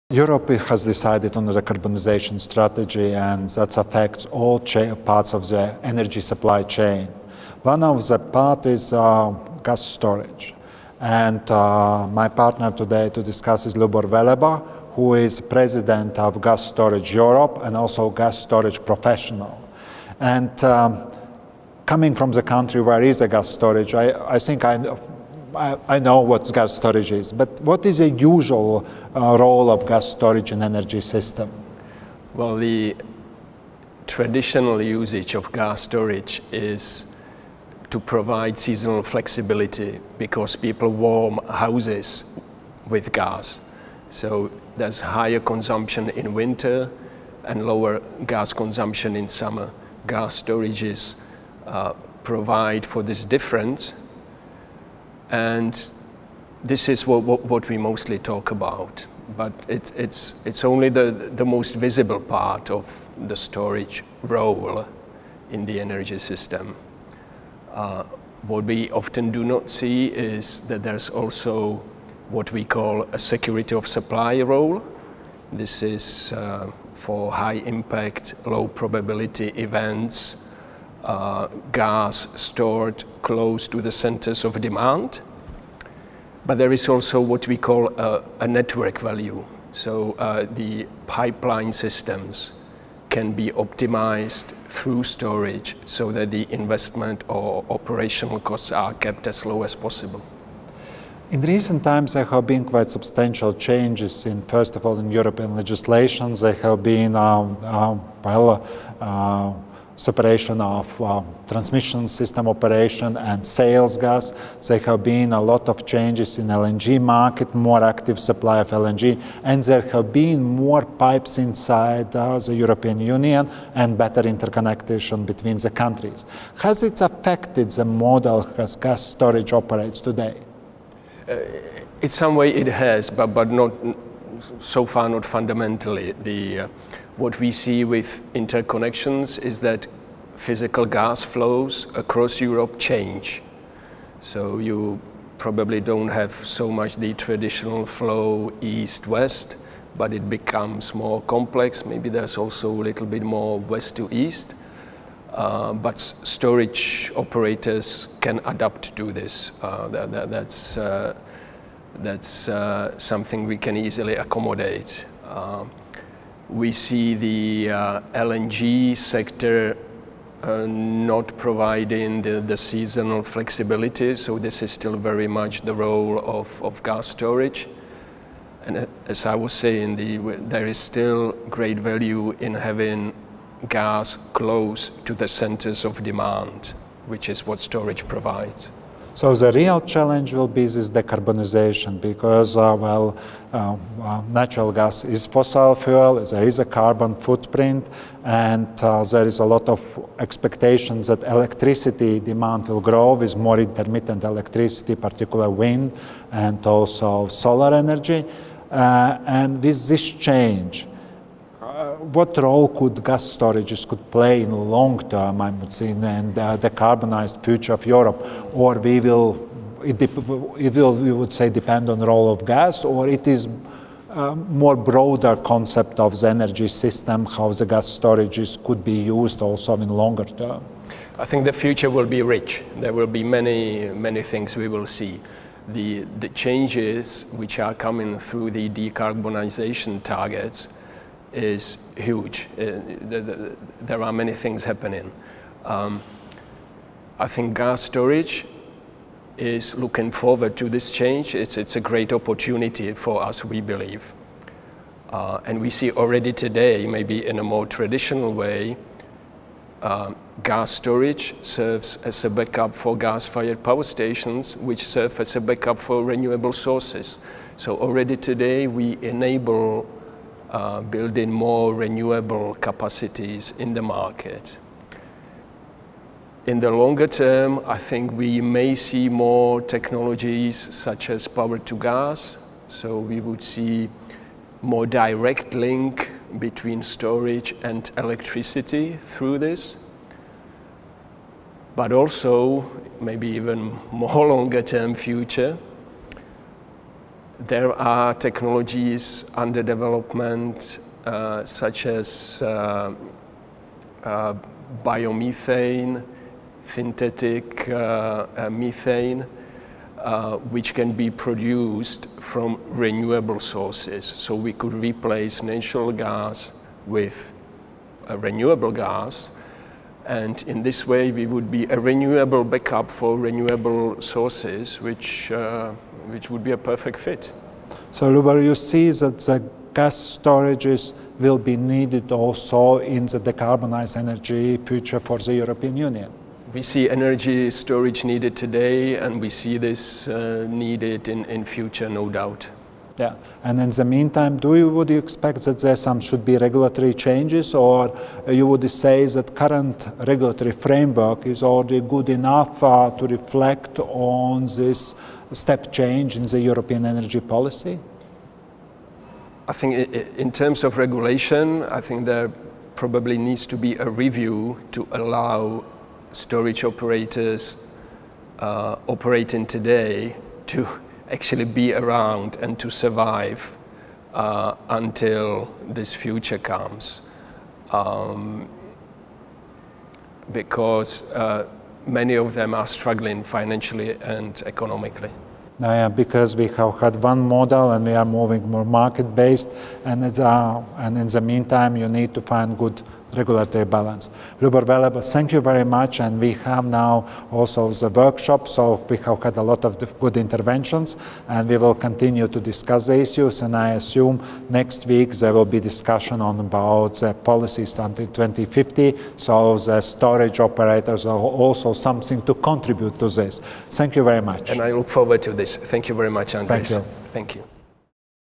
The Value of Energy Storage in Supporting EU’s Security of Supply and Decarbonisation Goals
On the margins of the FSR – Gas Infrastructure Europe (GIE) joint workshop held on the 6th of July 2018 in Florence, Andris Piebalgs (FSR) and Lubor Veleba (GIE) discuss the value of storage in supporting the long-term EU’s climate and energy objectives.

For decades, the gas storage played a significant role in the EU energy system not only as a source of seasonal flexibility, but also by providing security of supply and system value. However, the role of natural gas and gas storage will be challenged in deeply decarbonized scenarios. Lubor Veleba shares his ideas for the long-term decarbonized future with the renewable gas substituting the natural gas, and explains why the regulatory changes may be needed to support it.